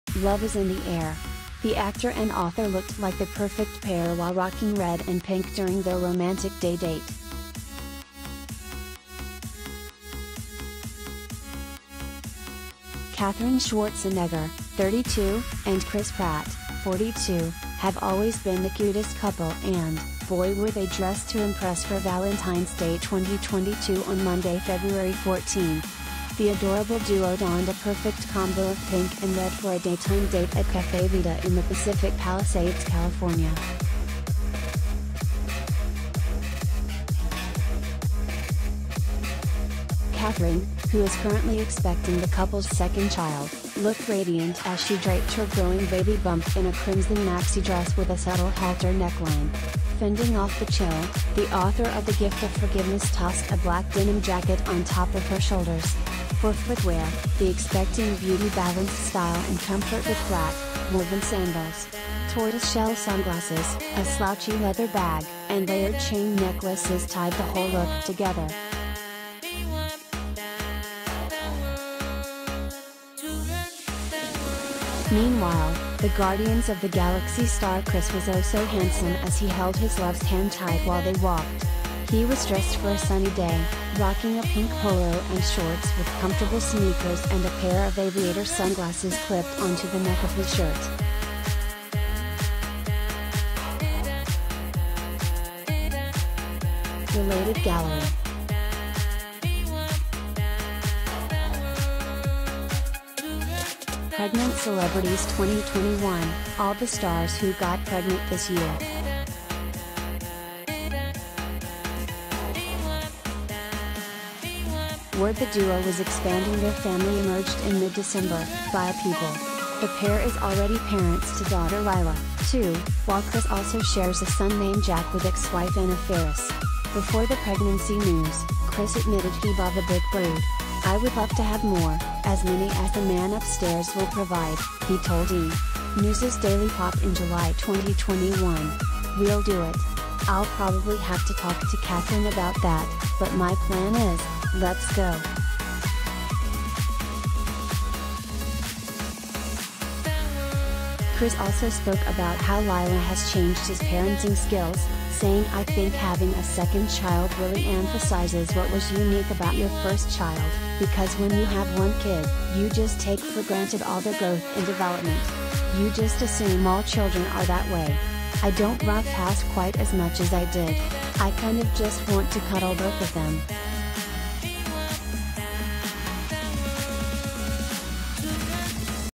Katherine Schwarzenegger Hold Hands With Chris Pratt On Valentine’s Day Date Ahead Of Baby No. 2
Love is in the air! The actor and author looked like the perfect pair while rocking red and pink during their romantic day date.
Katherine Schwarzenegger, 32, and Chris Pratt, 42, have always been the cutest couple and, boy were they dressed to impress for Valentine’s Day 2022 on Monday, Feb. 14. The adorable duo donned a perfect combo of pink and red for a daytime date at Cafe Vida in the Pacific Palisades, Calif.
Katherine, who is currently expecting the couple’s second child, looked radia